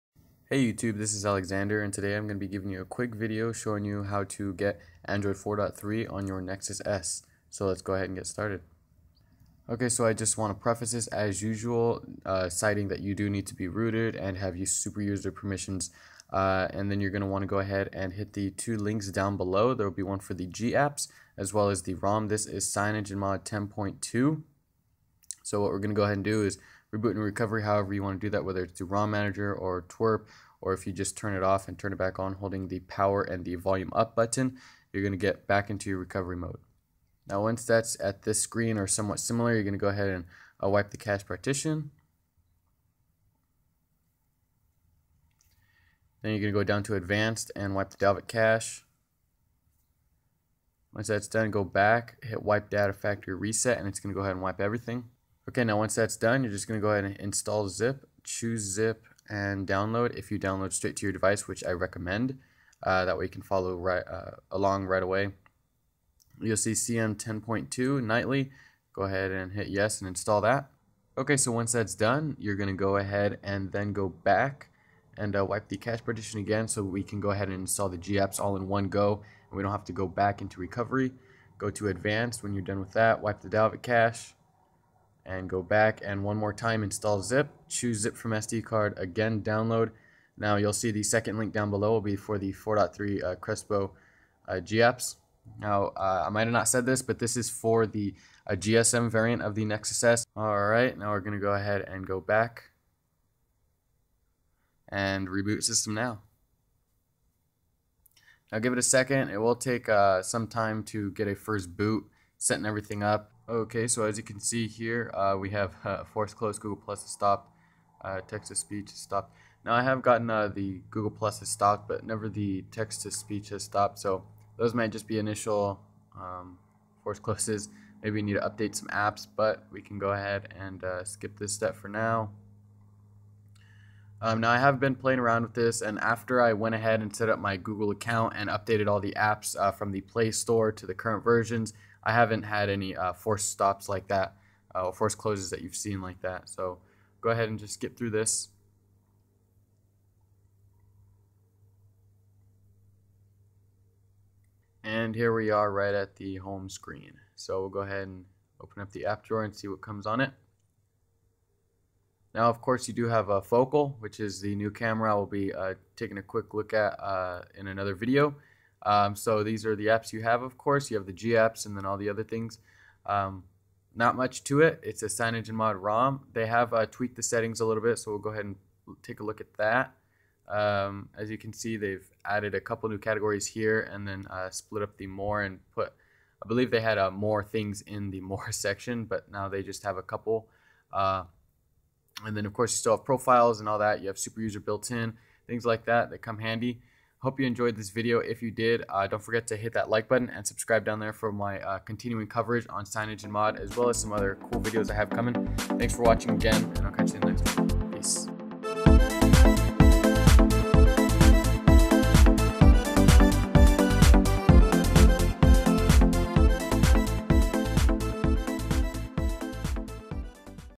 How to Install Cyanogenmod 10.2 (Android 4.3) on GSM Nexus S (Crespo)
Here's a quick video detailing how to install the new CM 10.2 for your GSM Nexus S.

Issues:
- Can't change the lockscreen shortcuts (they revert back to default)
- Focal/Camera FC sometimes